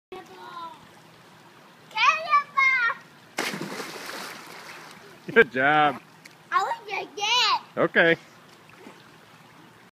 Cannonball!
Caden doing a cannonball lol